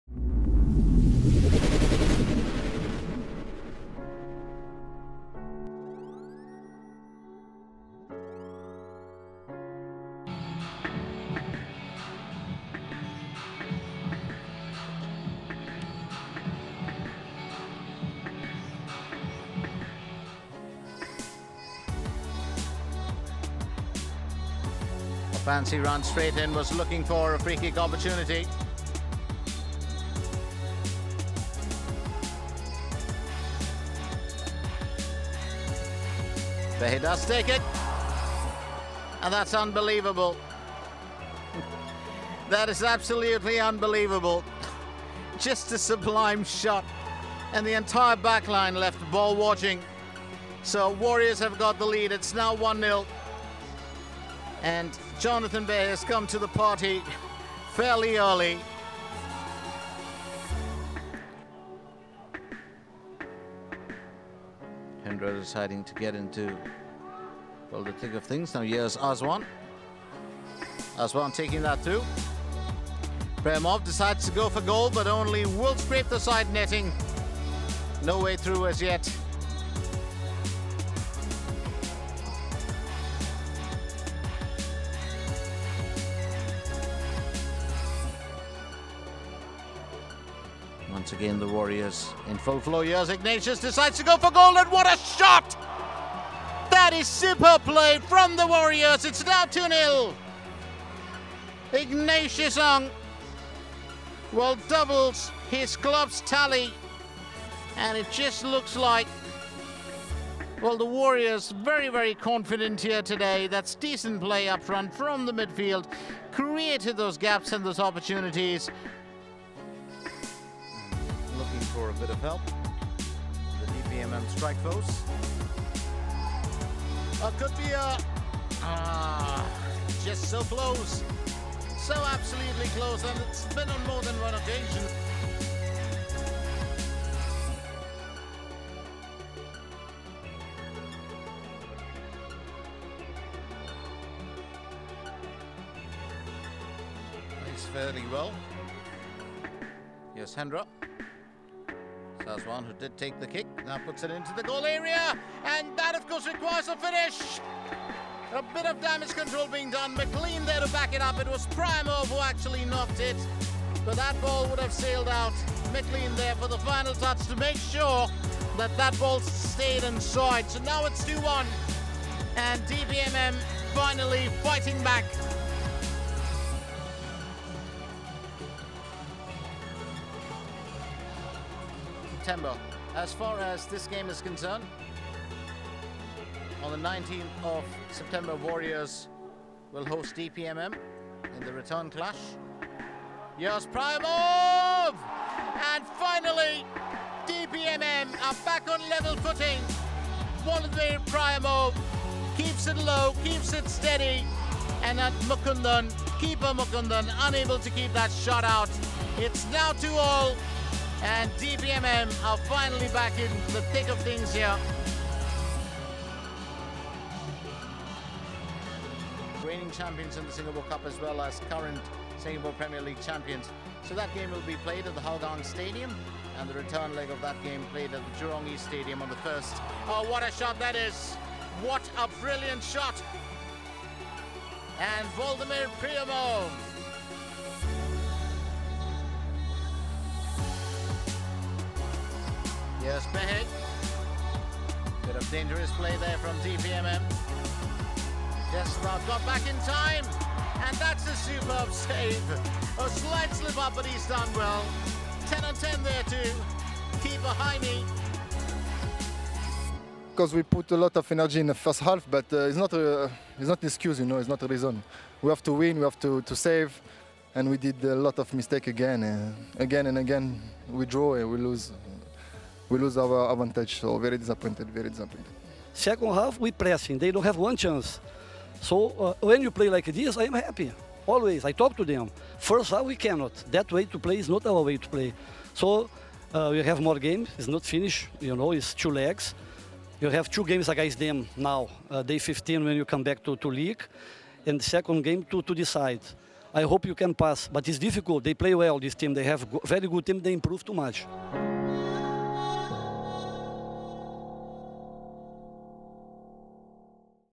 2018 Singapore Cup: Brunei DPMM FC 2-2 Warriors FC
First Round, First Leg
Venue: Jalan Besar Stadium
Scorers: Brian McLean 65’, Volodymyr Pryiomov 84’ | Jonathan Behe 8’, Ignatius Ang 35’